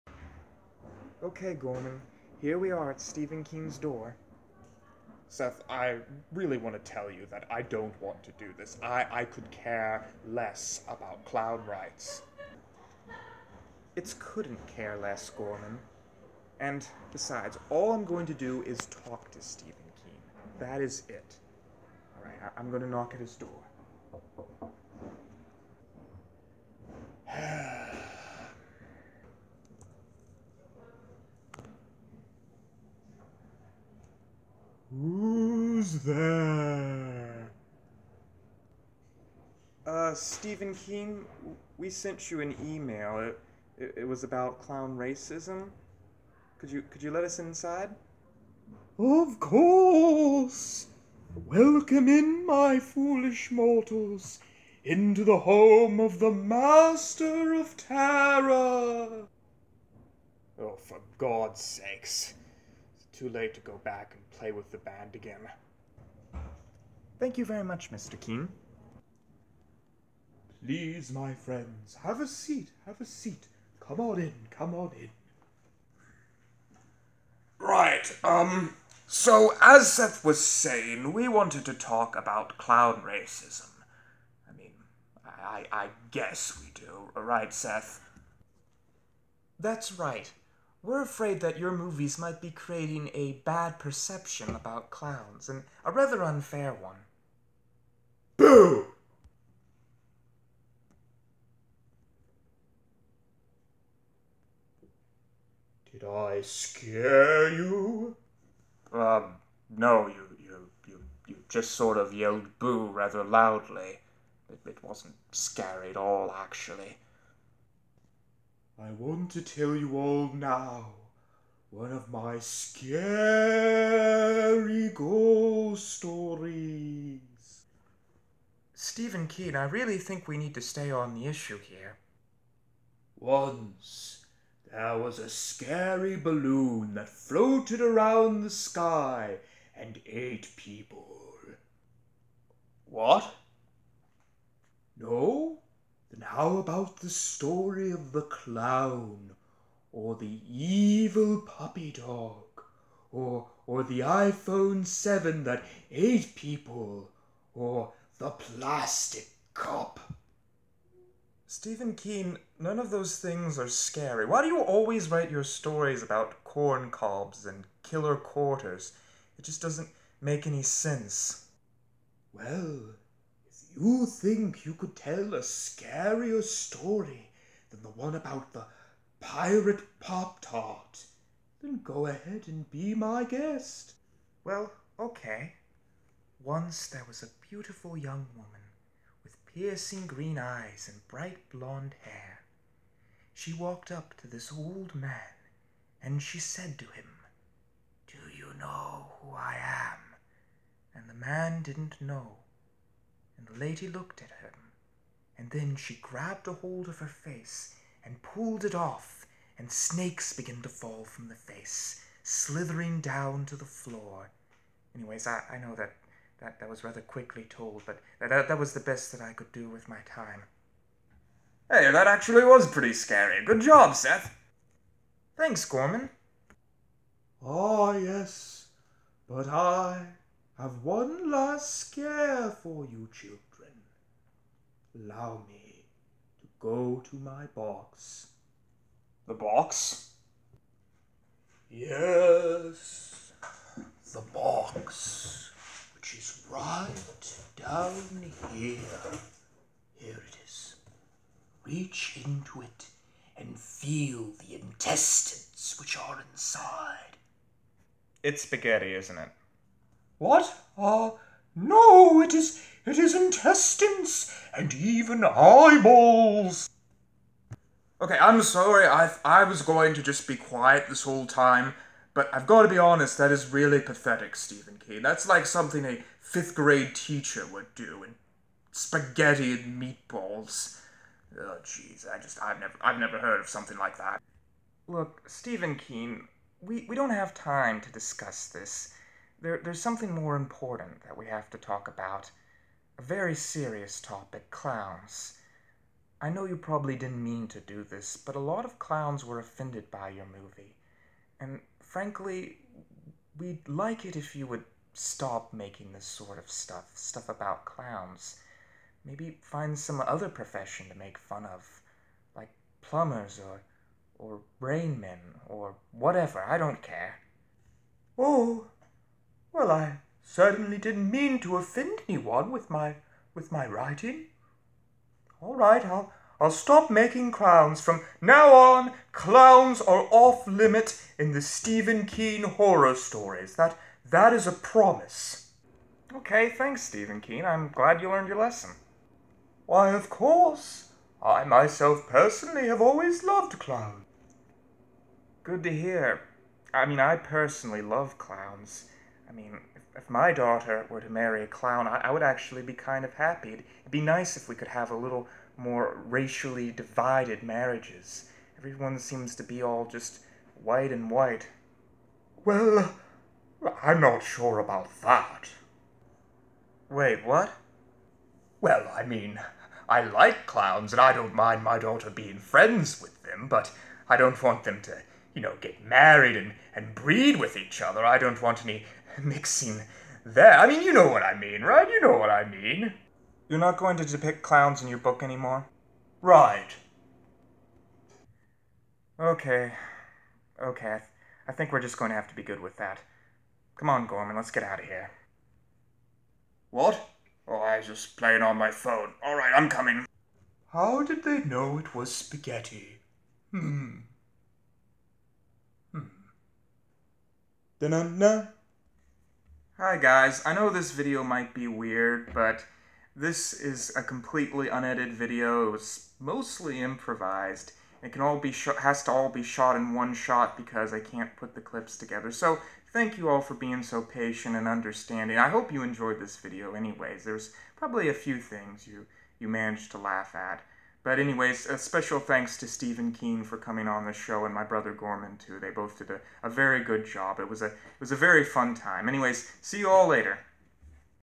Seth Meets Stephen King
Episode #256. Seth meets Stephen King the horror author and tries to talk him out of using clown stereotypes. Warning: The following episode had to be created without the Youtube editor and with only one shot. It also had to be improvised. If you are okay with that please watch on. Understand that this is the only way I can make video's now. My apologies.